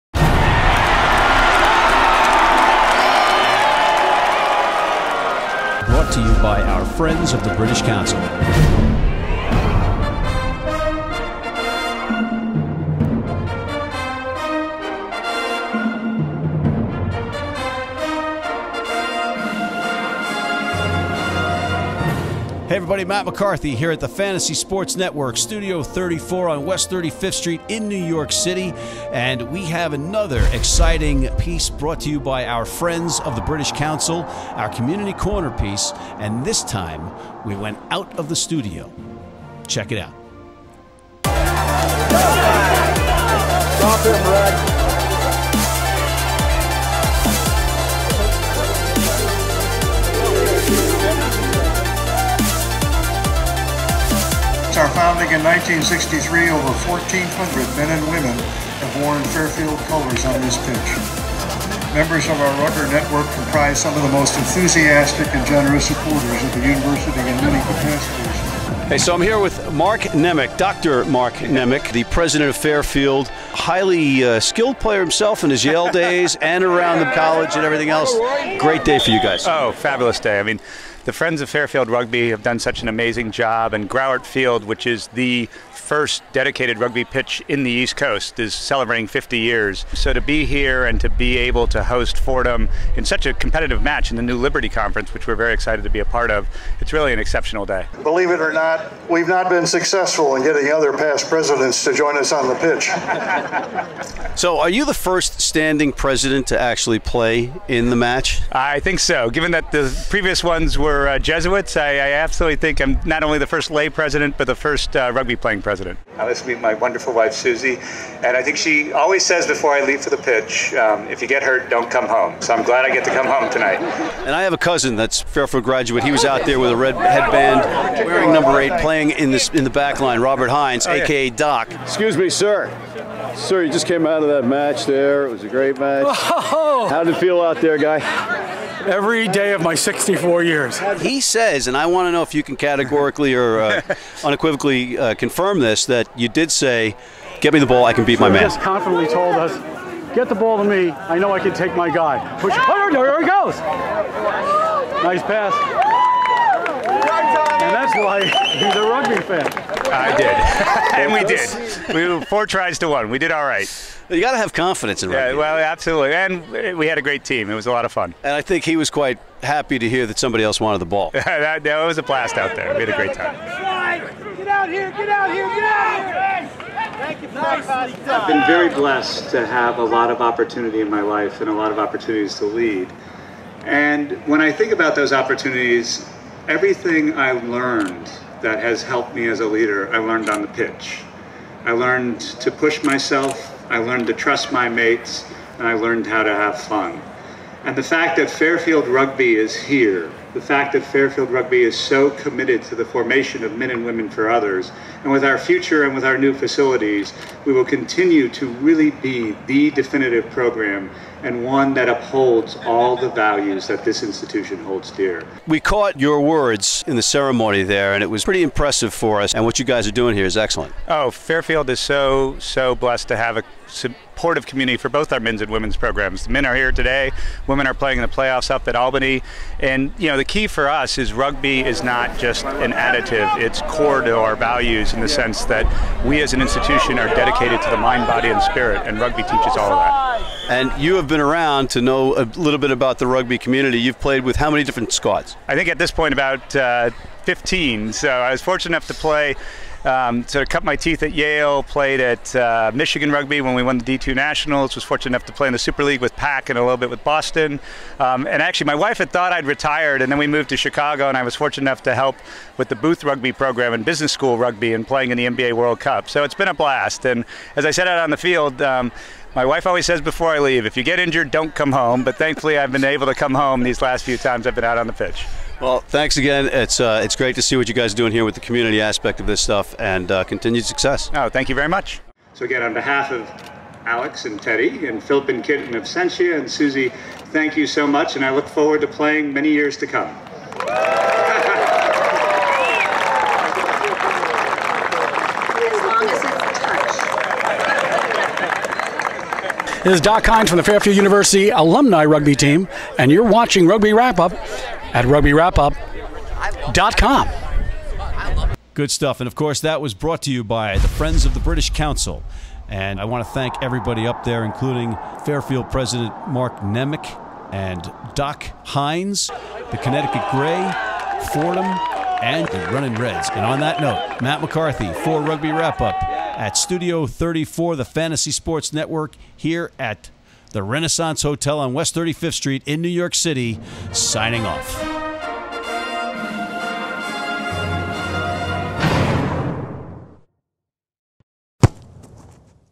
Community Corner: Fairfield's Mark Nemec, Dock Hynes, Fordham, Ct Grey
With the support of The Friends of the British Council, we're able to spend time with grassroots, club and college programs. At Fairfield University, all came together on a glorious day of rugby. Our Matt McCarthy was there when Fairfield University President Mark Nemec took the pitch along with 64-year-old center Robert "Dock" Hynes, against the vaunted Old Boys squad that is the Connecticut Grey. McCarthy got both on camera, along with nuggets from the marquee match between Fairfield and Fordham.